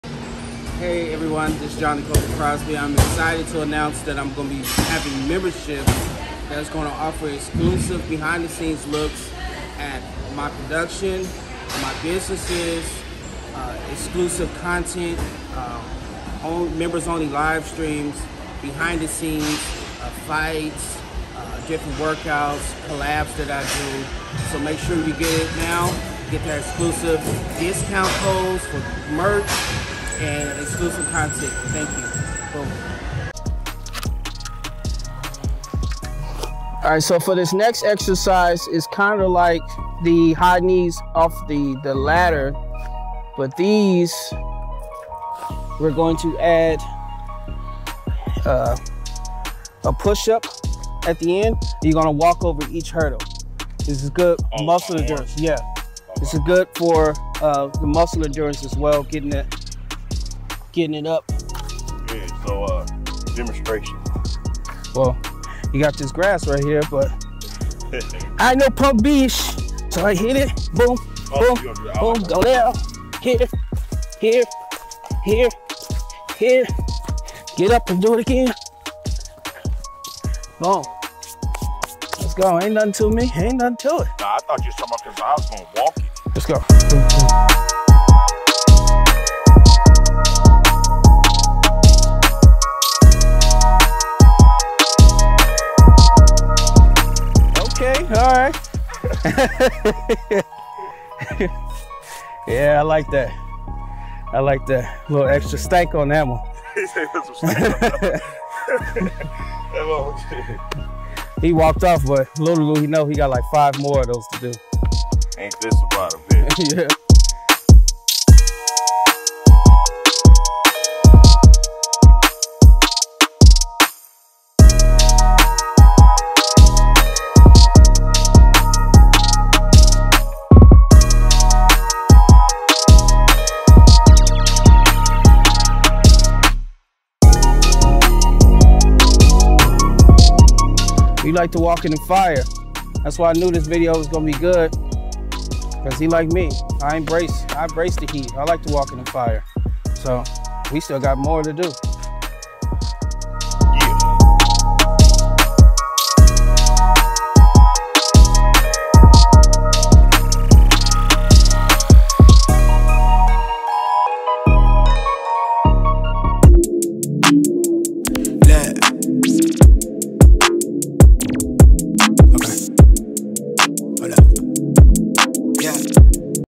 Membership announcement
New membership for my channel not only helps the channel but offers exclusive content. Its 3 levels each level opens up new perks. Thank you in advance

For shirts and other merchandise